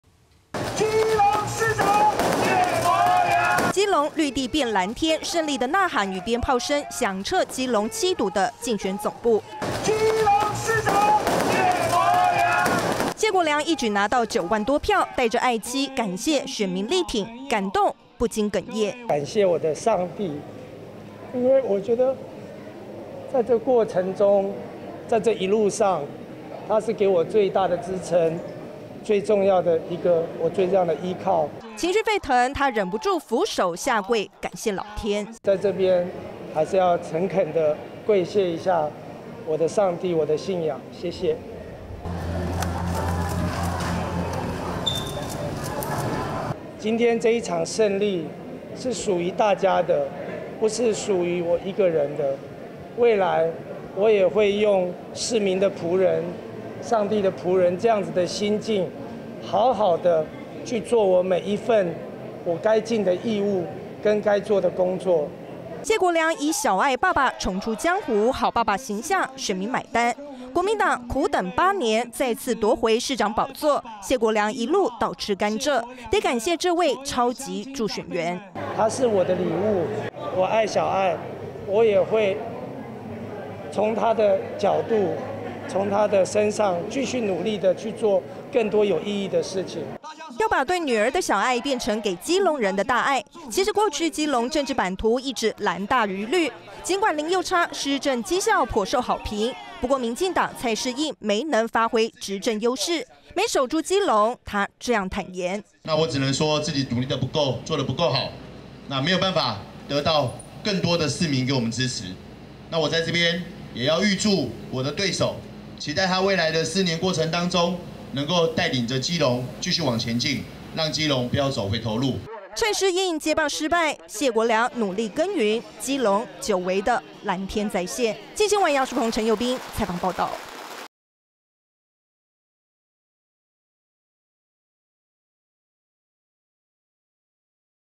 基隆綠地變藍天！ 謝國樑哽咽、激動下跪謝天｜鏡新聞
來看基隆選戰「綠地變藍天」，國民黨的基隆市長當選人謝國樑得票數超過9萬票，昨晚11/26他帶著妻子到競選總部感謝選民支持，他發表感言時，更是激動哽咽，說這一路上帝給他最大的支柱，最後甚至當場跪地謝天長達8秒！而落敗的蔡適應 ，沒能守住民進黨執政優勢，他坦言自己不夠努力，並且恭喜謝國樑，希望他帶領基隆、繼續向前行，不要讓基隆走回頭路。